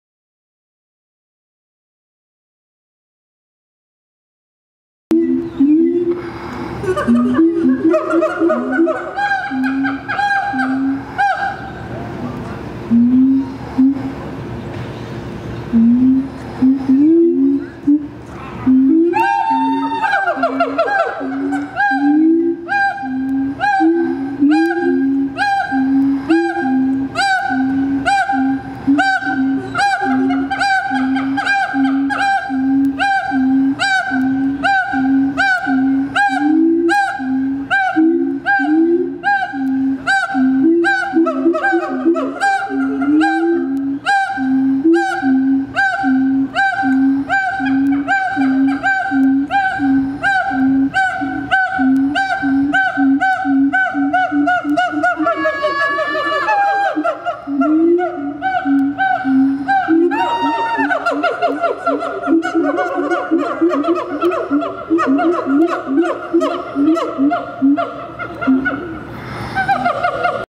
Siamang Duet Song
We visited the zoo on Sunday to capture as many of the animals on film as we could.  Meet two of the siamangs at the Riverbanks Zoo.  They are the largest species in the gibbon family and are on the endangered species list.  The siamangs have a duet song that identifies their territory.  The song is made up of loud booms and barks, amplified by resonating sounds across their inflated throat sacs. Make sure the sound is on when viewing this!  [Information from the Riverbanks Zoo website.]

Video captured with a Canon 70D and a 100-400mm lens.